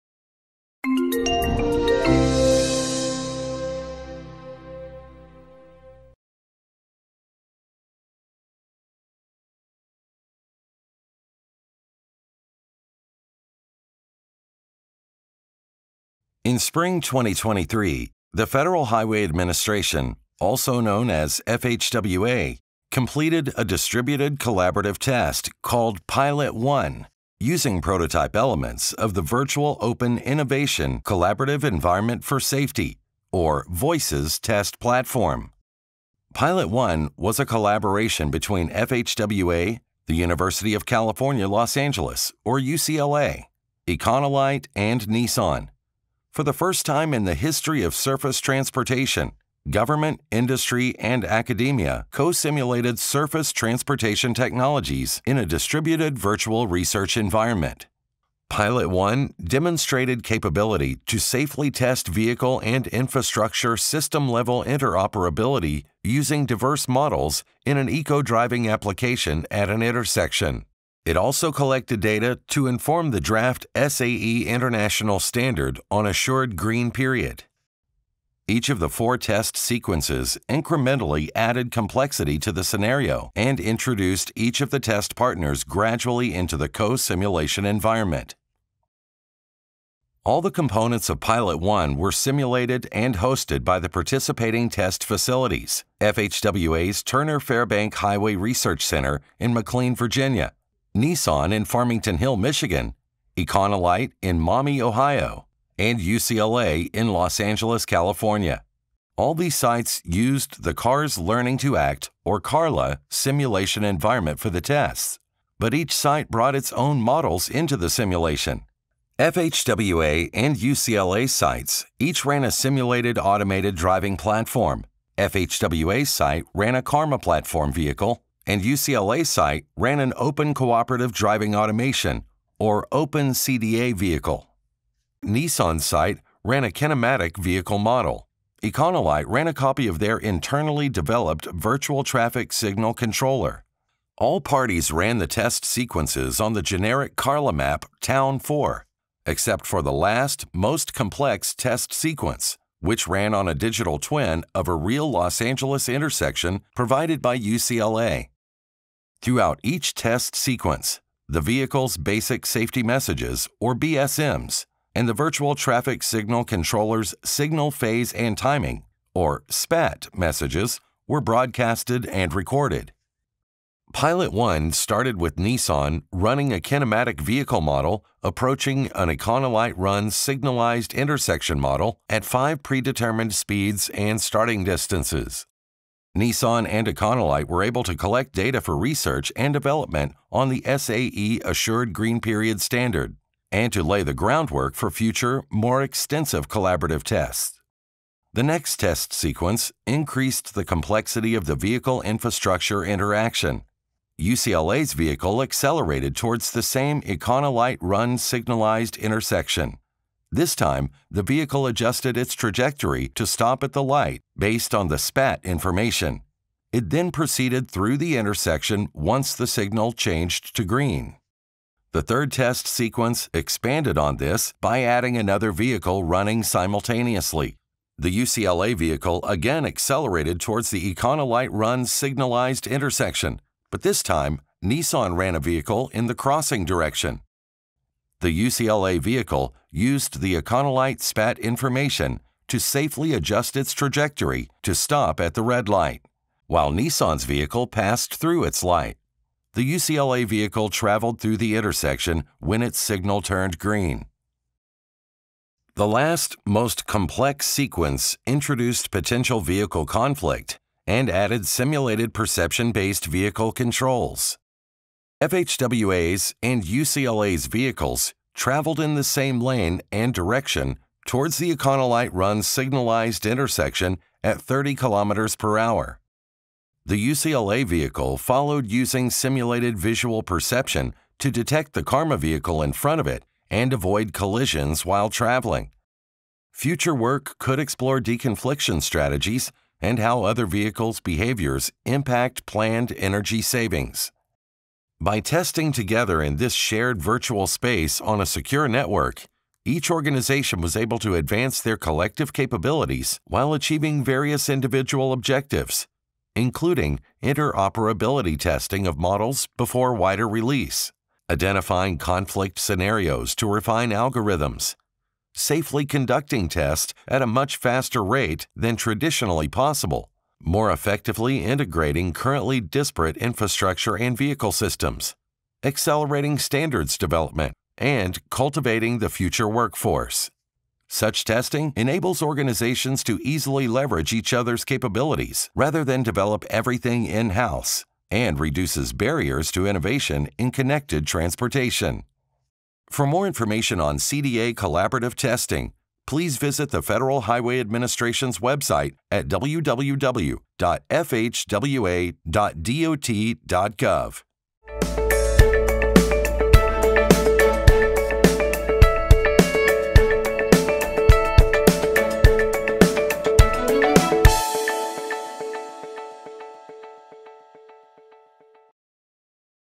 CDA Collaborative Pilot Test 1
This video describes CDA Collaborative Pilot Test 1, conducted between FHWA, the University of California, Los Angeles, or UCLA, Econolite, and Nissan in spring 2023.  For the first time in the history of surface transportation; government, industry, and academia co-simulated surface transportation technologies in a distributed virtual research environment.